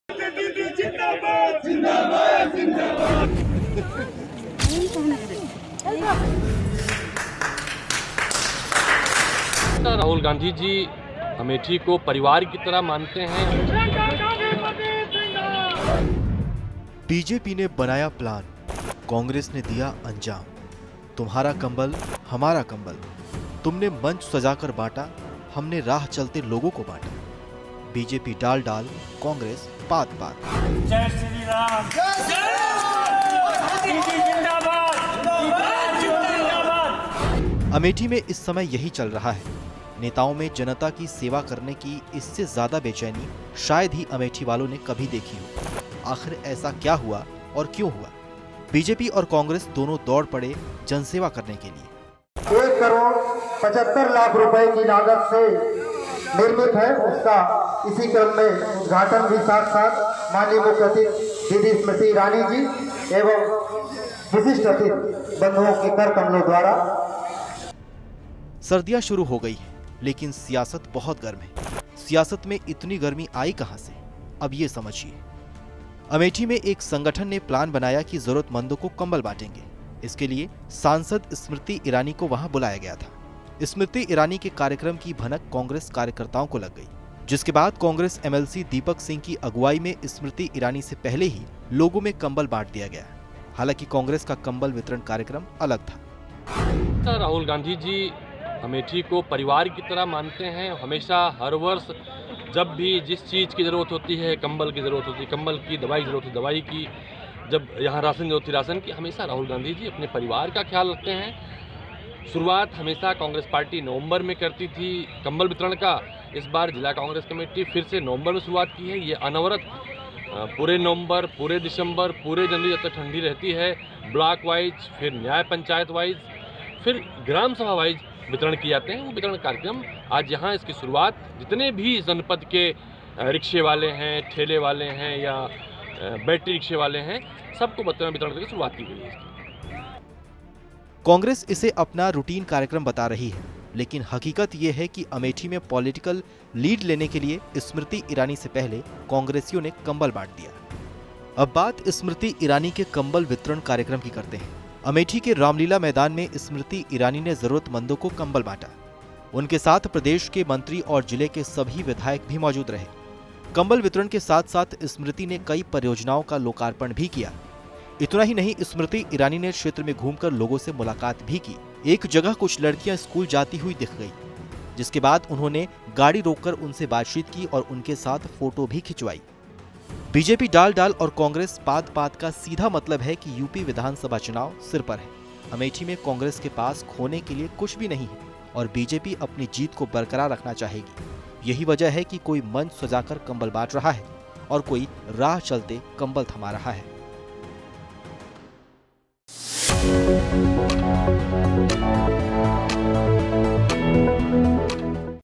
स्मृति और कांग्रेस की लड़ाई 'कंबल' पर आई
बीजेपी ने बनाया प्लान, कांग्रेस ने दिया अंजाम...तुम्हारा कंबल, हमारा कंबल.। तुमने मंच सजाकर बांटा, हमने राह चलते लोगों को बांटा...बीजेपी डाल डाल कांग्रेस पात पात
अमेठी में इस समय यही चल रहा है.,...नेताओं में जनता की सेवा करने की इससे ज्यादा बेचैनी शायद ही अमेठी वालों ने कभी देखी हो..। आखिर ऐसा क्या हुआ औऱ क्यों हुआ बीजेपी और कांग्रेस दोनों दौड़ पड़े जनसेवा करने के लिए...सर्दियां शुरु हो गई हैं, लेकिन सियासत बहुत गर्मी है..।  सियासत में इतनी गर्मी आई कहां से? अब ये समझिए...
अमेठी में एक संगठन ने प्लान बनाया कि, जररूतमंदों को कंबल बांटेंगे, इसके लिए सांसद स्मृति ईऱानी को वहां बुलाया गया था..।  स्मृति ईरानी के कार्यक्रम की भनक कांग्रेस कार्यकर्ताओं को लग गई... जिसके बाद कांग्रेस एमएलसी दीपक सिंह की अगुवाई में स्मृति ईऱानी से पहले ही लोगों में कंबल बांट दिया गया.. हालांकि कांग्रेस का कंबल
वितरण कार्यक्रम अलग था..कांग्रेस इसे अपना रूटीन कार्यक्रम बता रही है, लेकिन हकीकत ये है कि, अमेठी में पॉलिटकल लीड लेने के लिए स्मृति ईरानी से पहले कांग्रेसियों ने कंबल बांट दिया..अब बात स्मृति ईरानी के कंबल वितरण कार्यक्रम की करते हैं। अमेठी के रामलीला मैदान में स्मृति ईऱानी ने जरूरतमंदों को कंबल बांटा, उनके साथ प्रदेश सरकार के मंत्री, और जिले के सभी विधायक भी मौजूद थे.,, कंबल वितरण के साथ साथ स्मृति ने कई परियोजनाओं का लोकार्पण भी किया. । इतना ही नहीं स्मृति ईरानी क्षेत्र में घूमकर लोगों
से मुलाकात भी कि... एक  जगह कुछ लड़कियां स्कूल जाती हुई दिख गई, जिसके बाद उन्होंने गाड़ी रोककर उनसे बातचीत की और उनके साथ फोटो भी खिंचवाई...बीजेपी डाल डाल कांग्रेस पात पात का सीधा सा मतलब है कि, यूपी विधानसभा चुनाव सिर पर है, अमेठी में कांग्रेस के पास खोने केलिए कुछ भी नहीं है, औऱ बीजेपी अपनी जीत को बरकरार रखना चाहेगी, यही वजह है कि, कोई मंच सजाकर कंबल बाट रहा है, कोई राह चलते कंबल थमा जा रहा है ।LIVE TV देखने के लिए इस लिंक पर क्लिक करें: